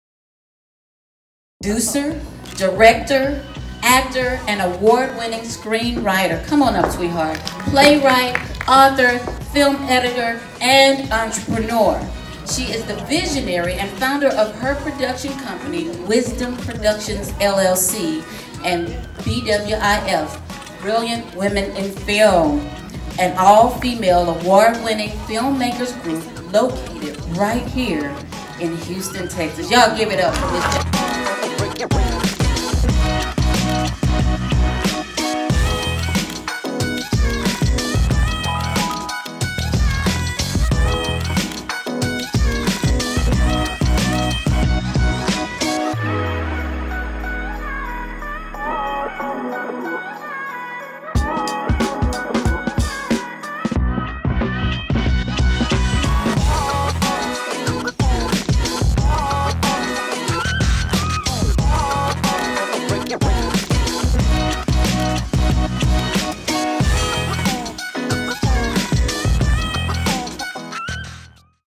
Author's Award Night
Joan Montreuil being awarded as an Author of 3 published books! Congratulations